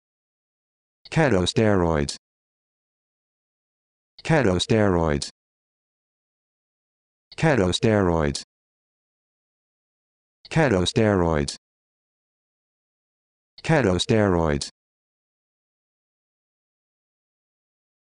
How to Pronounce Ketosteroids
This video shows you how to pronounce Ketosteroids